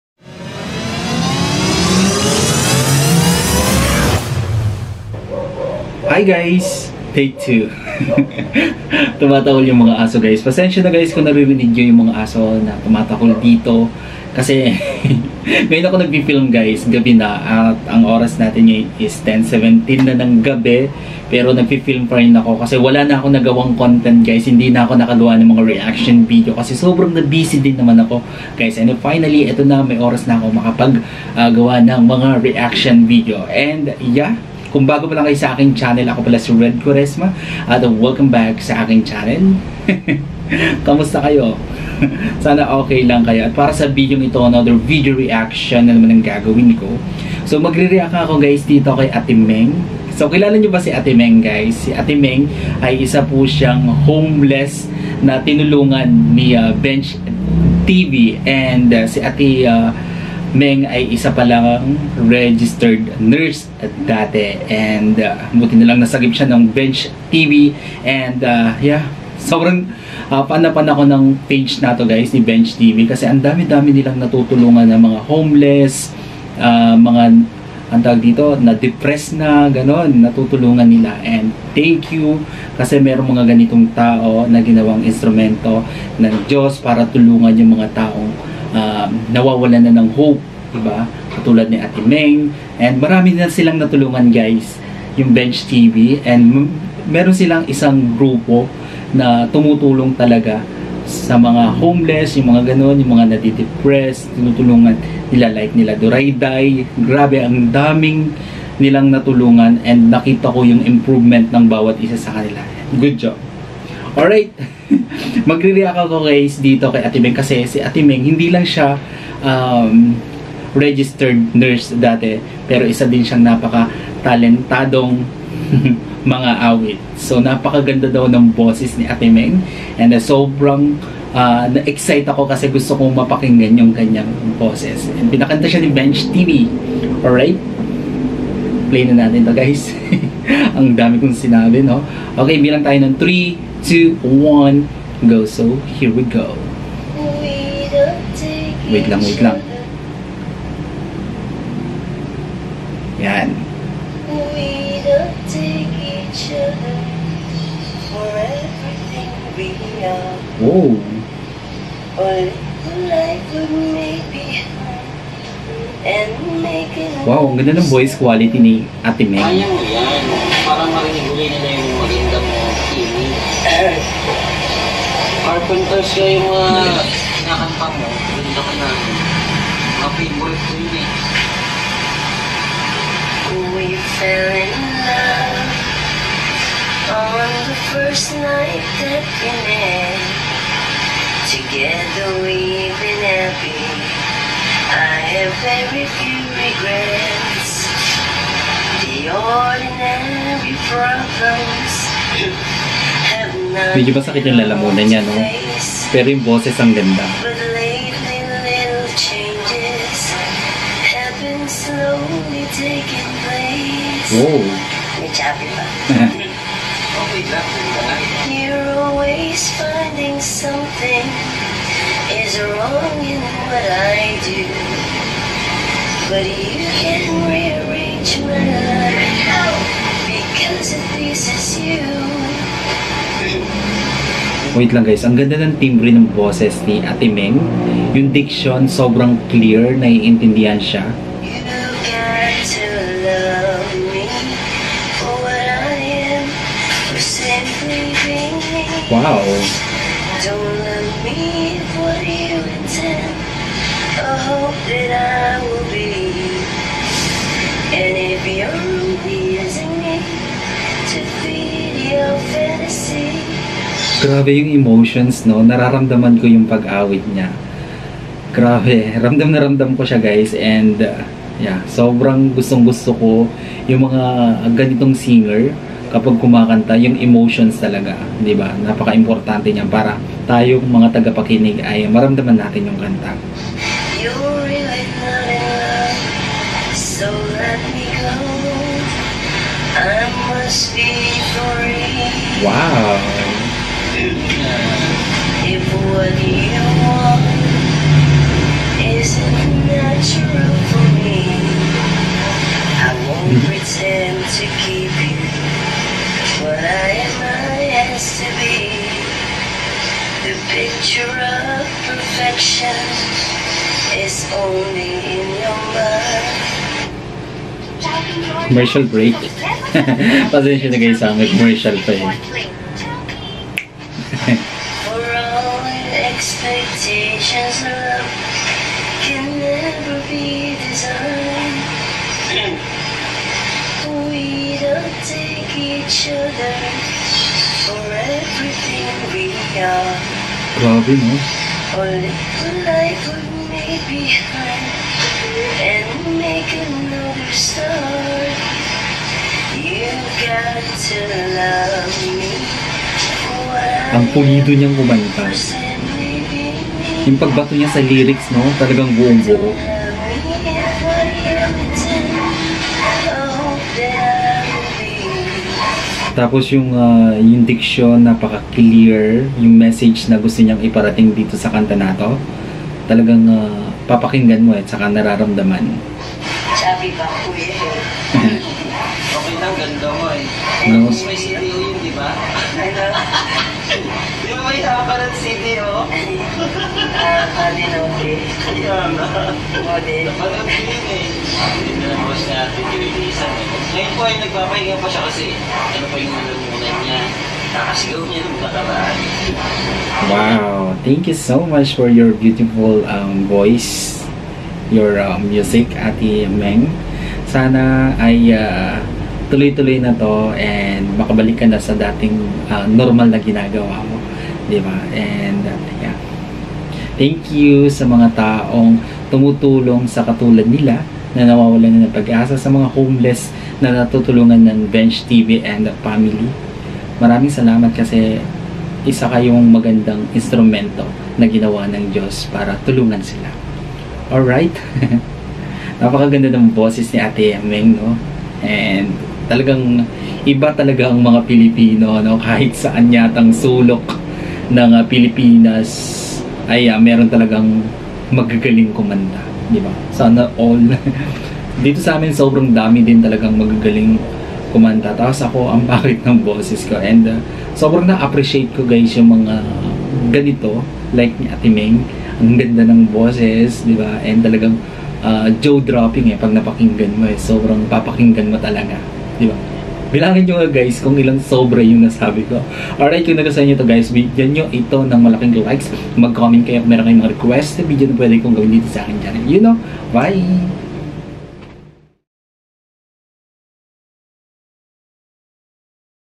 VIRAL HOMELESS SINGER AT DATING NURSE NA SI ATE MENG ANG GANDA PALA NG BOSES | REACTION
BENCH TV